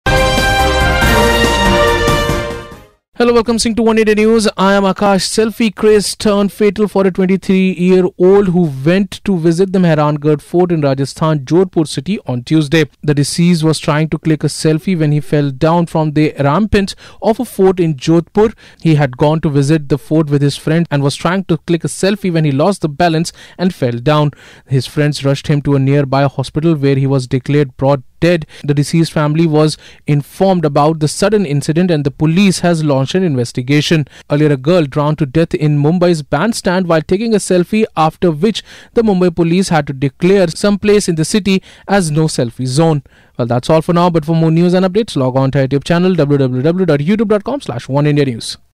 Youth falls to death while taking selfie at Mehrangarh Fort
Selfie craze turn fatal for a 23 years old, the tourist who went to visit the Mehrangarh Fort in Rajasthan's Jodhpur city on Tuesday. He was trying to click selfie when he fell down from the ramparts of the forts in Jodhpur. Earlier a girl drowned to death in Mumbai's Bandstand while taking a selfie after which the Mumbai police had to decalre some places in the city 'no selfie zone'.